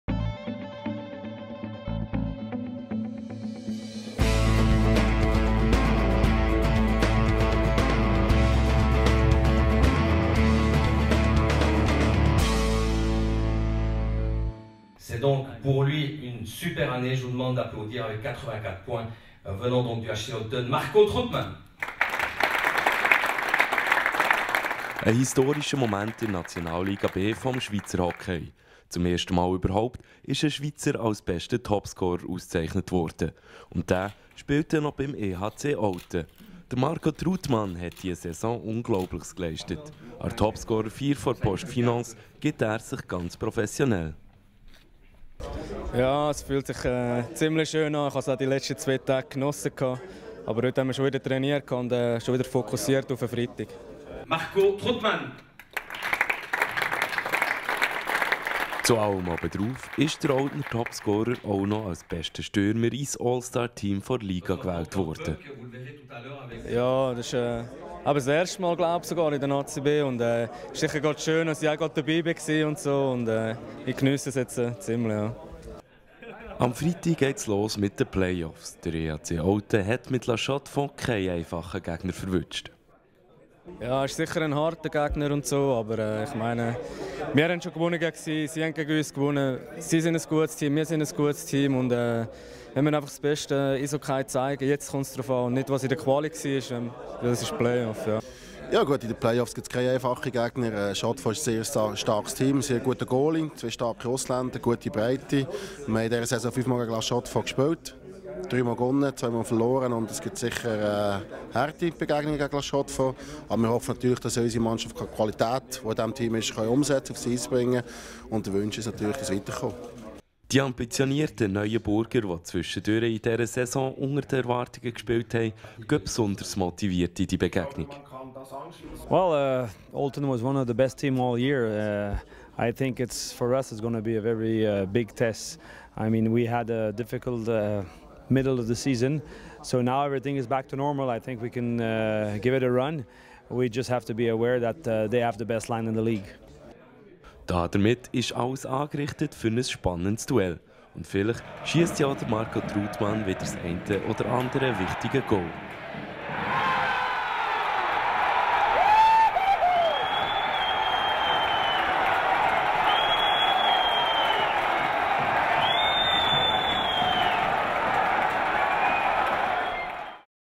EHCO TV - Doppelte Ehrung für Truttmann
Marco Truttmann wird als erster Schweizer als bester Topscorer der NLB ausgezeichnet und wurde ins Allstar-Team der Saison gewählt. Mit La Chaux-de-Fonds steht jetzt ein harter Gegner für die Play-offs an.